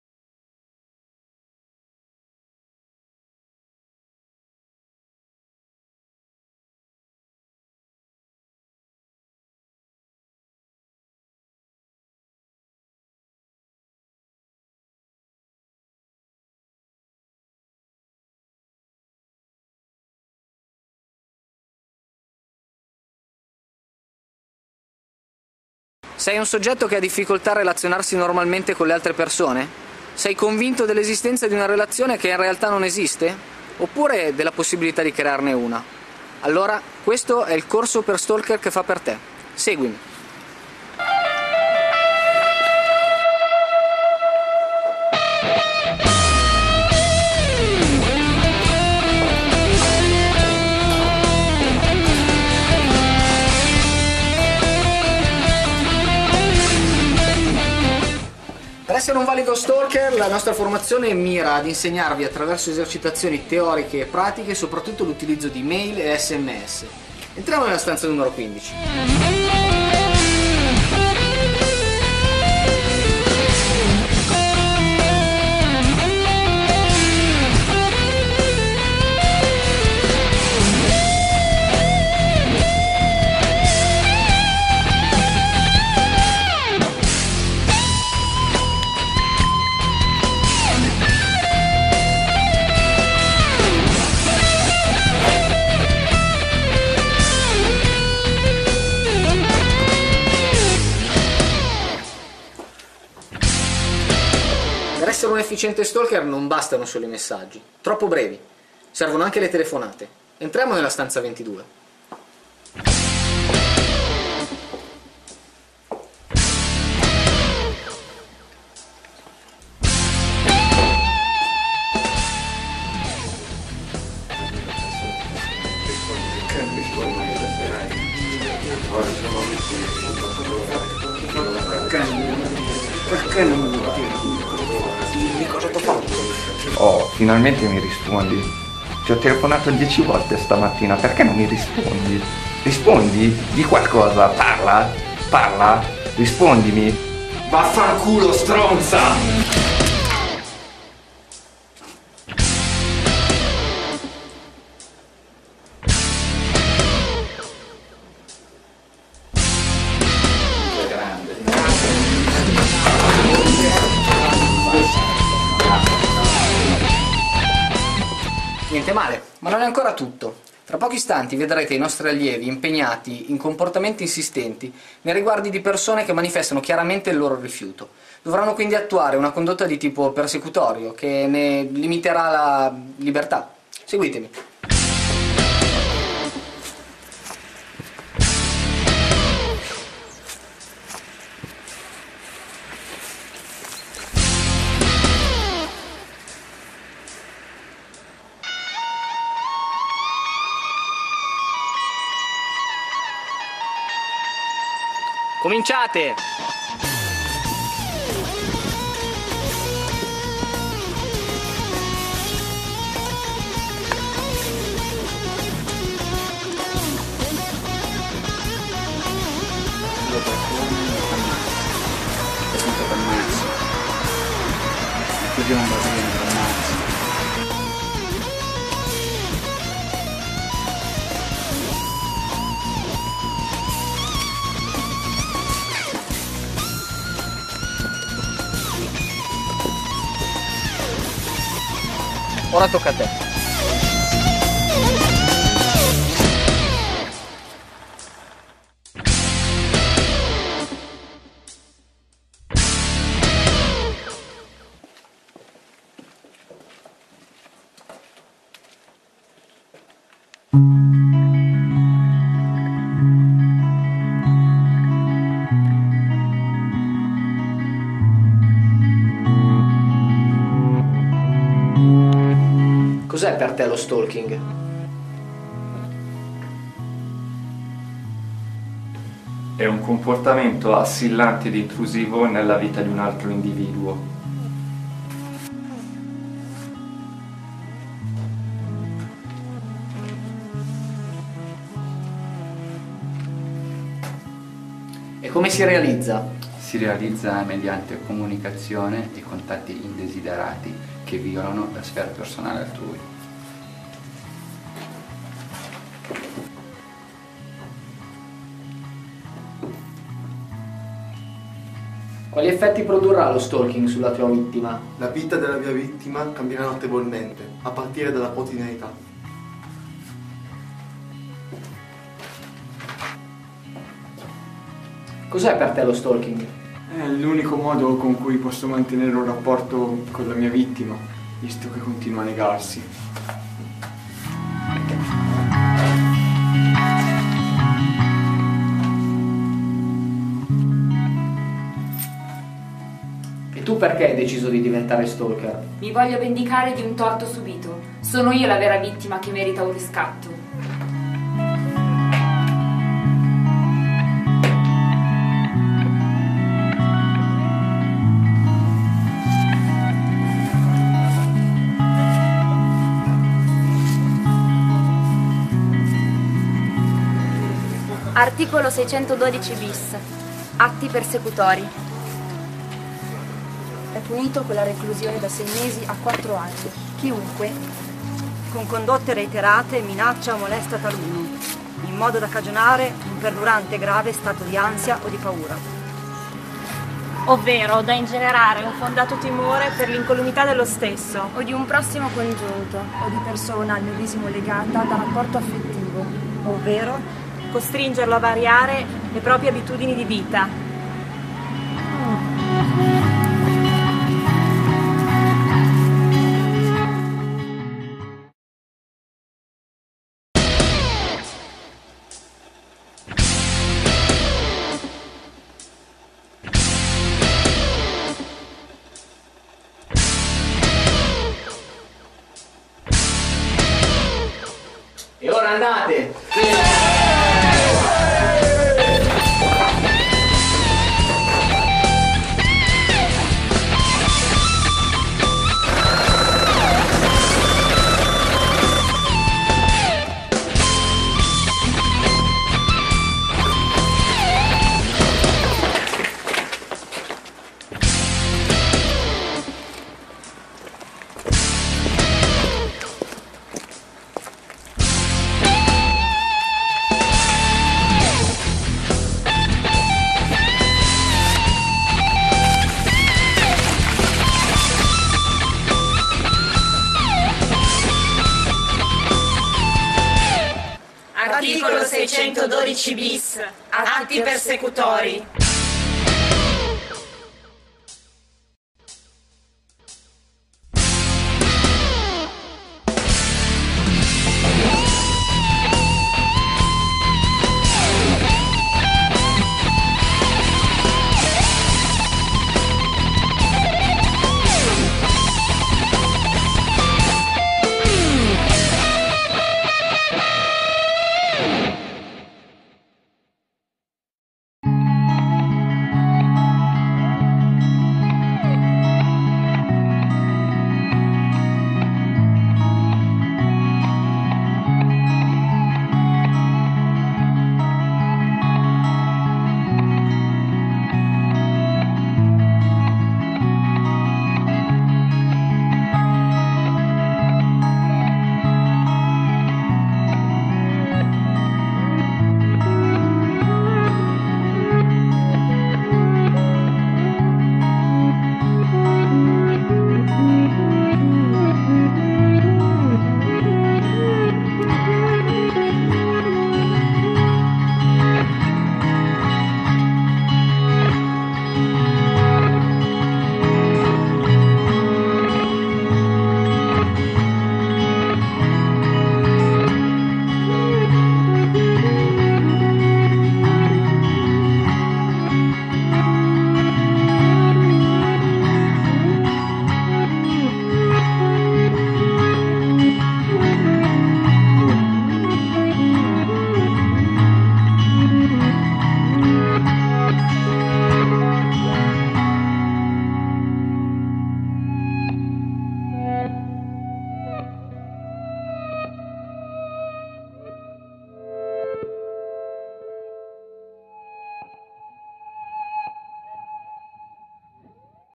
Corso di Formazione per Aspiranti Stalker
A video of Che need a far Reflect On the theme of Stalking On Women

Video vincitore della prima edizione
"DIAMO VOCE A CHI NON HA VOCE - SIMONA PER TUTTE"
Un video che serve a far riflettere sulla tematica dello Stalking sulle Donne